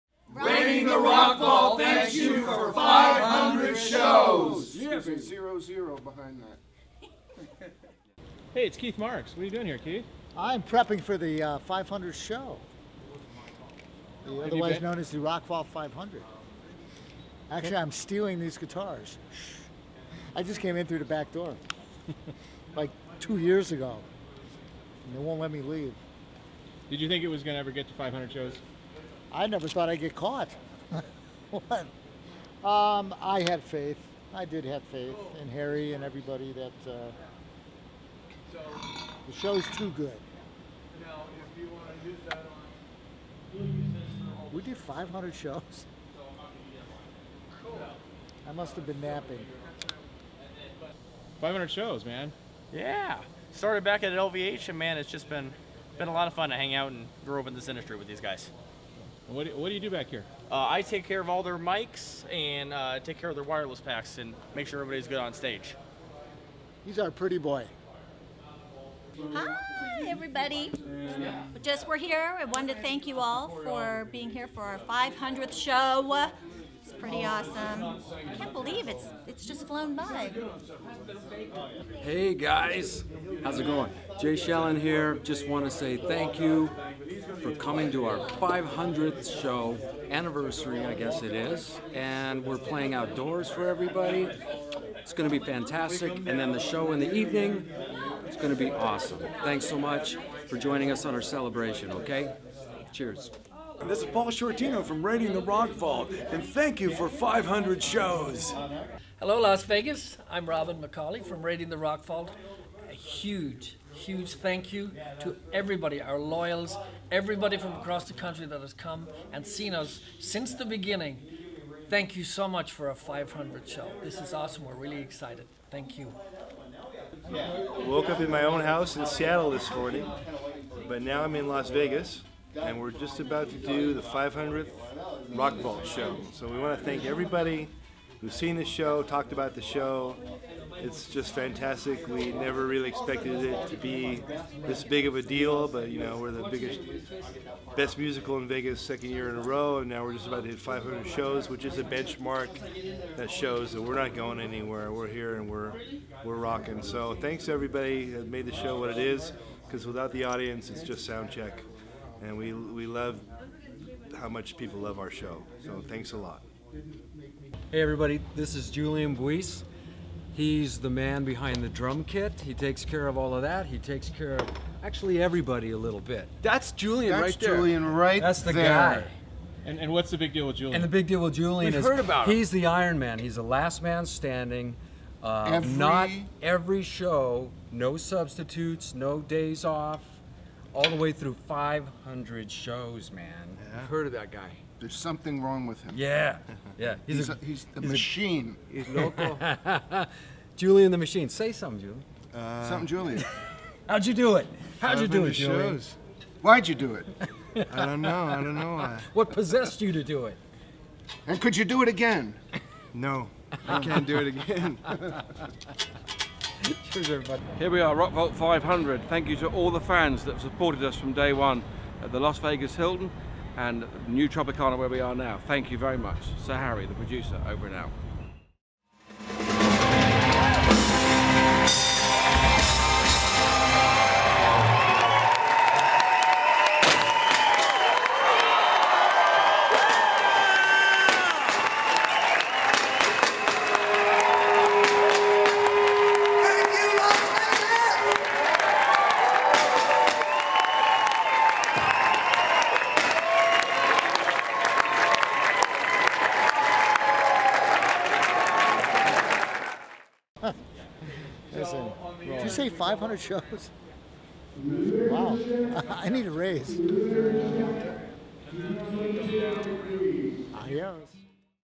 Thank you for 500 shows!
Thanks everyone who has supported Raiding the Rock Vault! We celebrated our 500th show May 29-31, 2015.